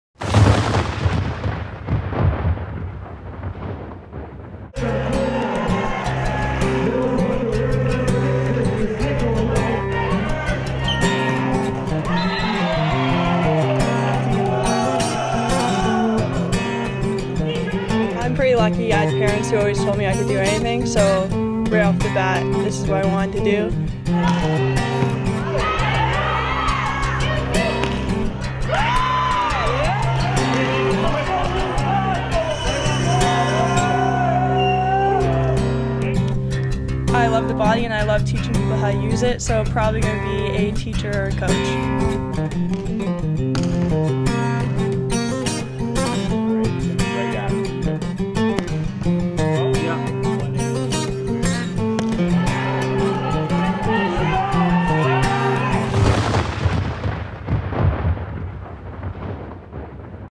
T-Bird Tell-All: Alissa Coulter
Alissa Coulter, of the UBC women's volleyball team, talks about her time as a T-Bird, her plans after UBC, and music